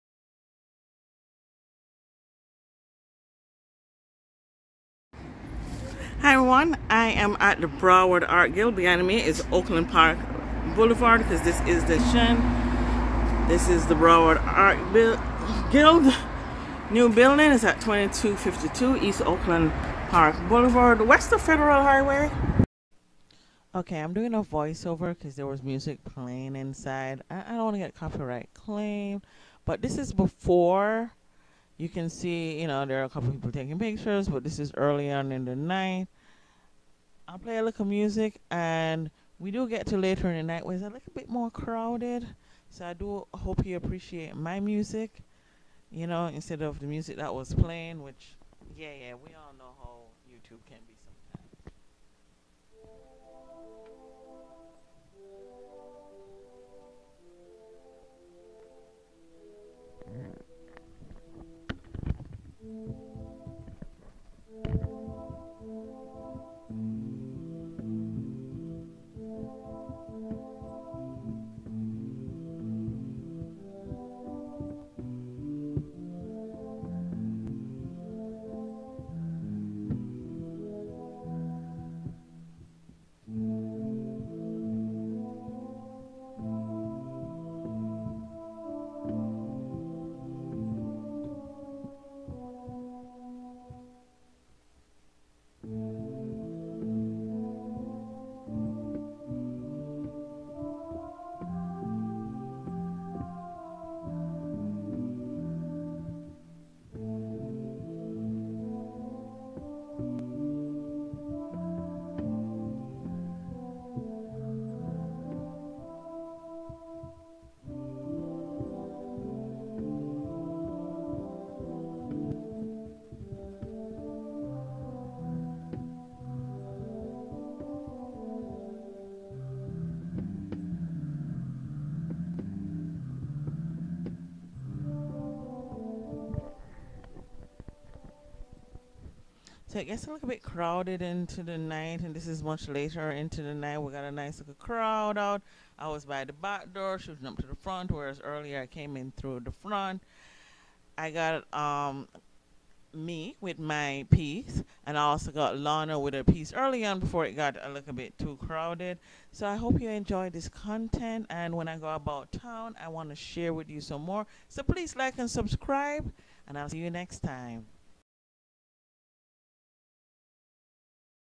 Art Squared Exhibit
Broward Art Guild's Art Square Exhibit was at the new gallery at 2052 E. Oakland Park Blvd. and runs until September 20, 2024. I volunteered for the Opening Reception on Saturday, September 7th and here are my highlights. Hi Lana! Hi Barb! Hi Gerard! Hi Gary! Hi Teena!